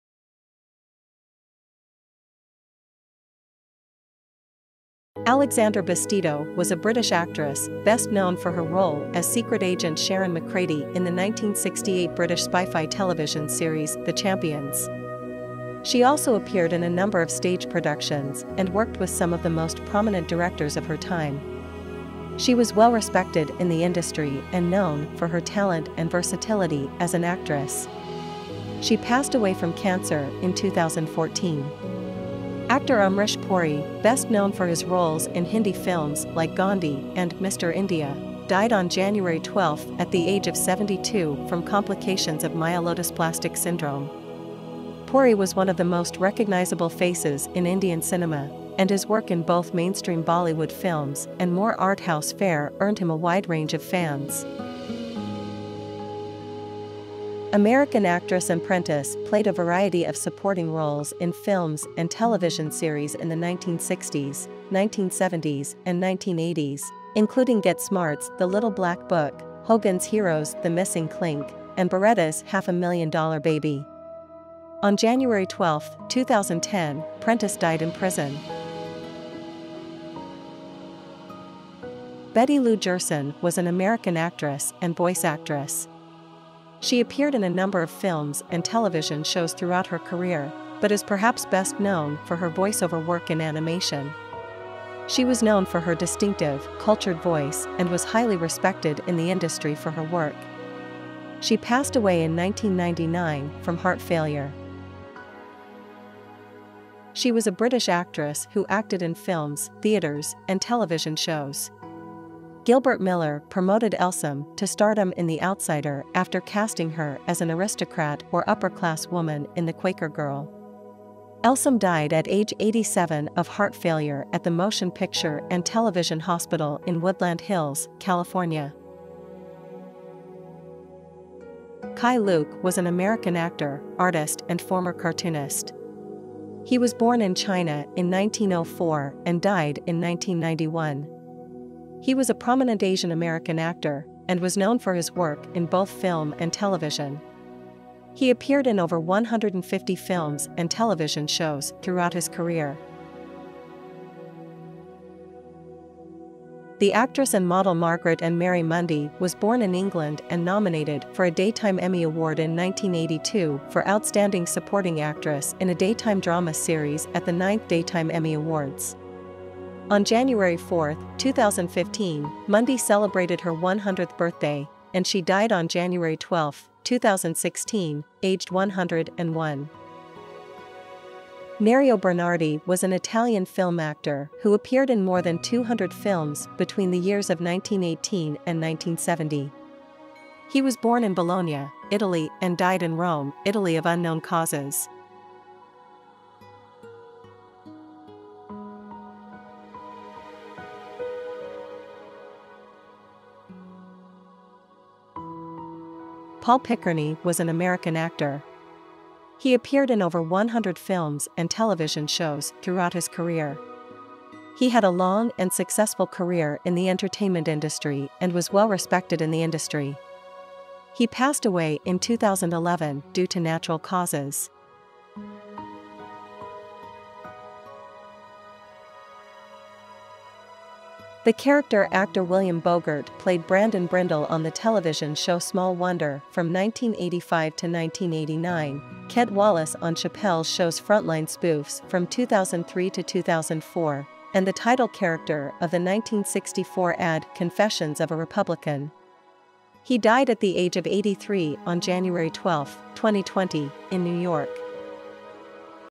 10 Famous Celebrities who died today 12th January - remembering big stars - 2023
Amrish Puri
William Bogert
Keye Luke
Betty Lou Gerson
Isobel Elsom
Meg Mundy
Paul Picerni
Alexandra Bastedo
Nerio Bernardi
Ann Prentiss